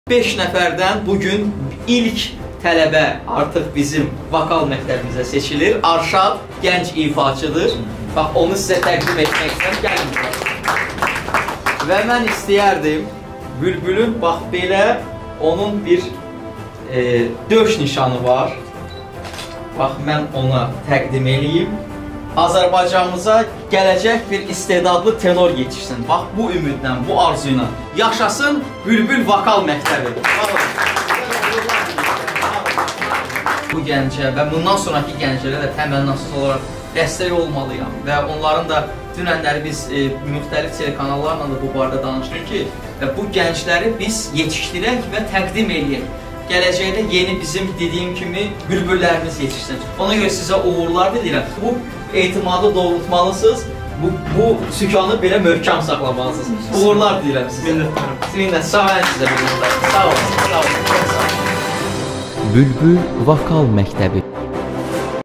Bülbül Vokal Məktəbinin ilk tələbəsi elan olundu
Azərbaycan Respublikasının Əməkdar artisti, tanınmış vokal ifaçısı Ramil Qasımov tərəfindən   yaradılan Bülbül Vokal Məktəbinin ilk tələbəsi elan olundu. Azərbaycan Milli Konservatoriyası nəzdində Musiqi Kollecində təhsil alan 19 yaşlı Ərşad Heydərov Bülbül Vokal Məktəbində təmənnasız təhsil alacaq.